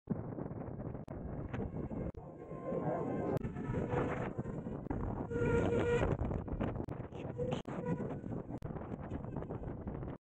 Who Will Knock The Begode Master Pro Off Of It’s Thrown?! Electric Unicycle
Rode the Begode Master Pro down the coast of Hampton Beach in New Hampshire.

Did some quick edits over @zil_euc (IG) amazing footage.